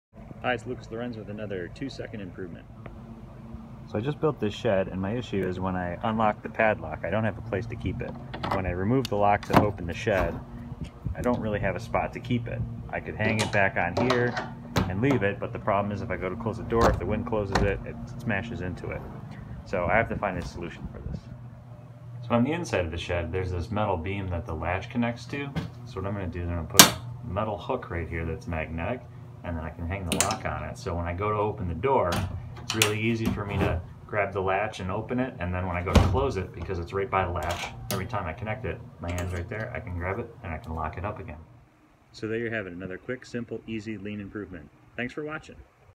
2 Second Shed
Simple 2 second improvement for my shed lock.